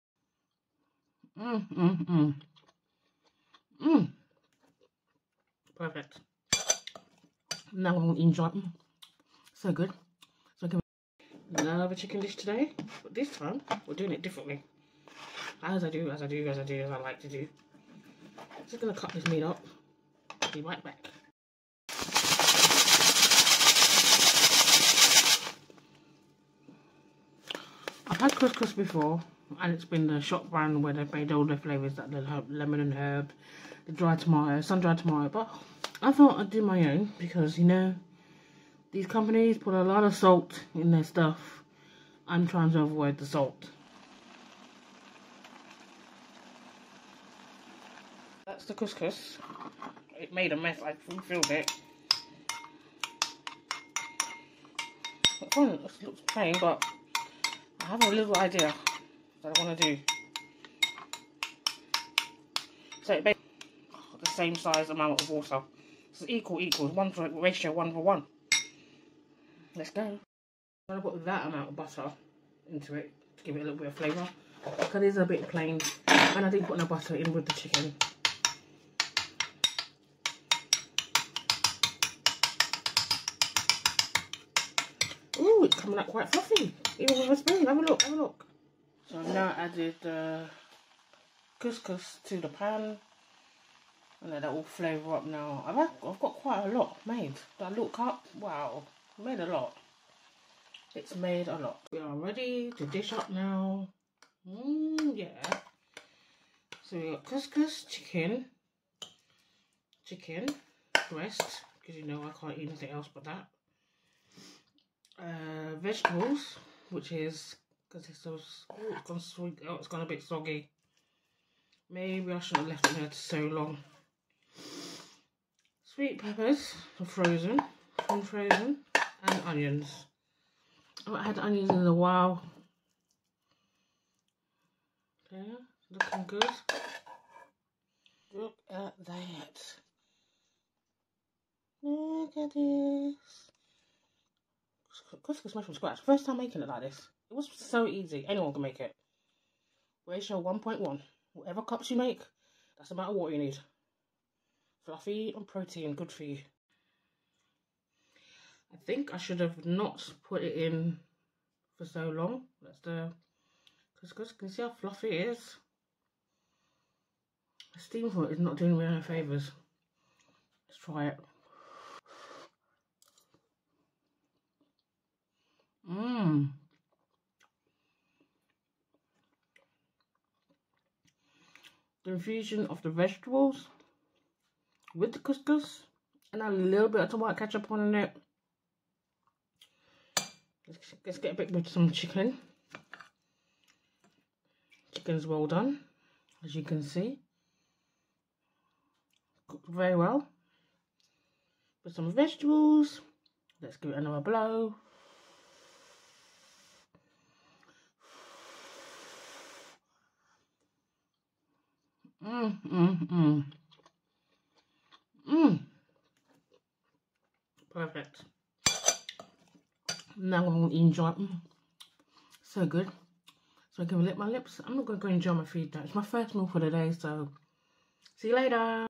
Just made this for the first time
Just made this dish for the first time Yummy Couscous with a few other ingredients makes a healthy dish pack with what you need on a weight loss journey